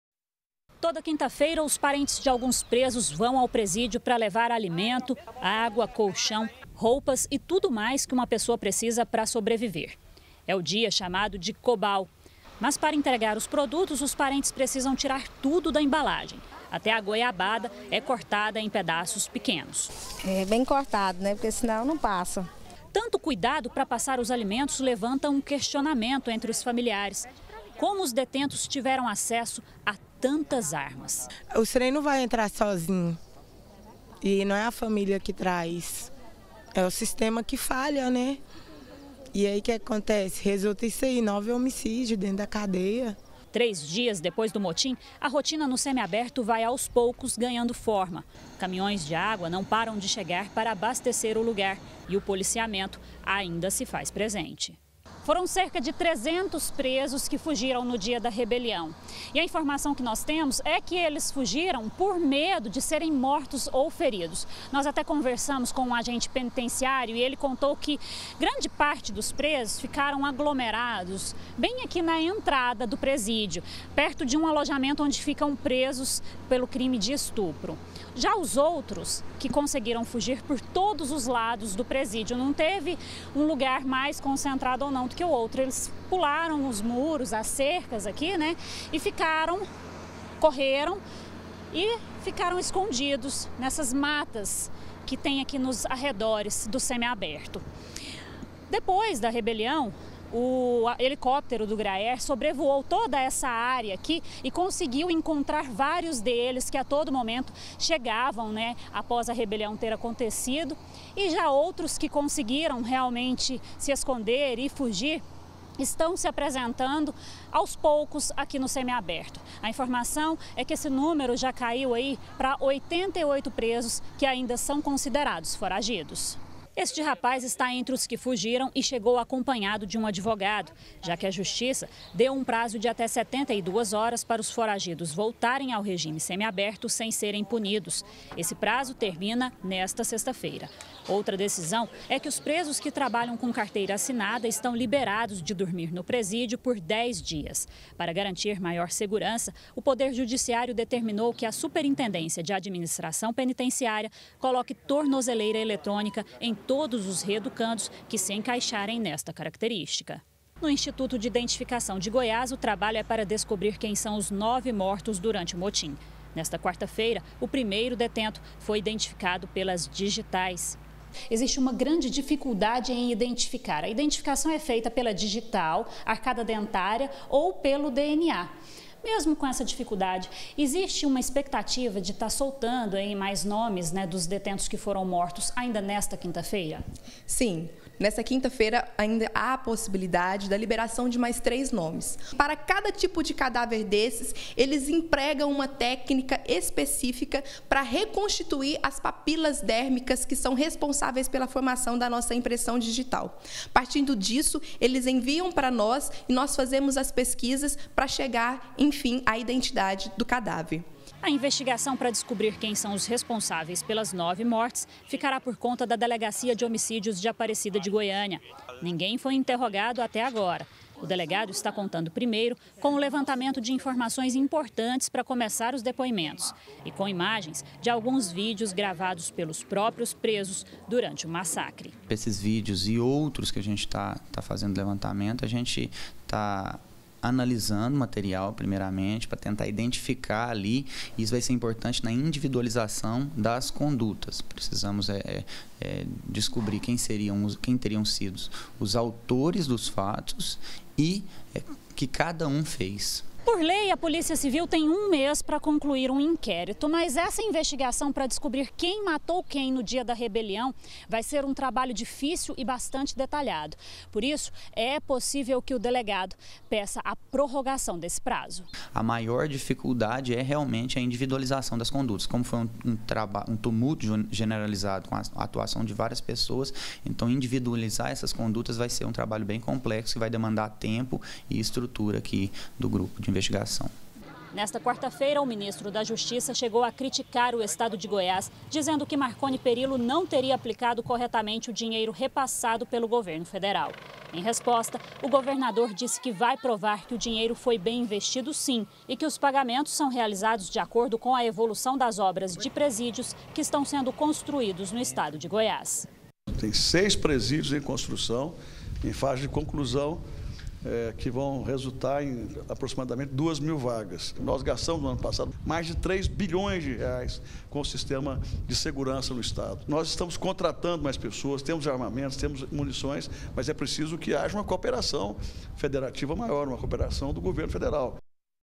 BG - Polícia investiga rebelião - 04-01-2018
A Polícia Civil tem 30 dias para concluir o inquérito sobre a rebelião que aconteceu no presídio do semi aberto. Mas este prazo pode ser estendido. 9 presos morreram. 88 ainda estão foragidos. Hoje foi dia dos parentes levarem comidas e outros objetos para os presos.